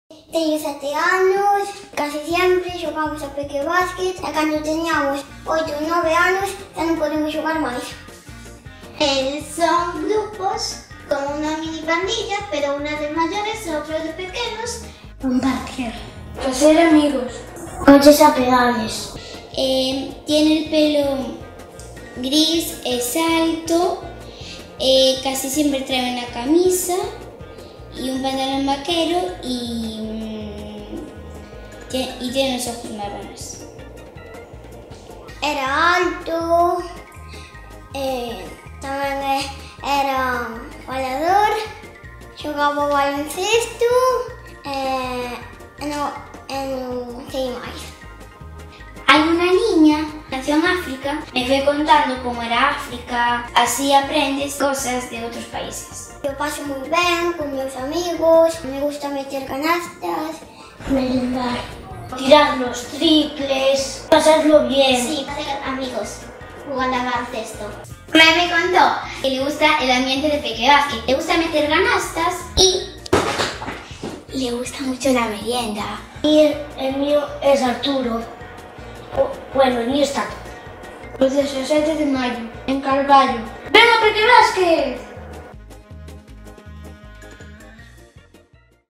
VIDEO ENTREVISTAS NIÑOS BALONCESTO PARA PEQUEBASKET CARBALLO
Vídeo entrevistas a niños que juegan a Baloncesto entre 5 y 8 años en Carballo, A Coruña, en el evento Pequebasket que reúne a más de 700 niños de toda Galicia.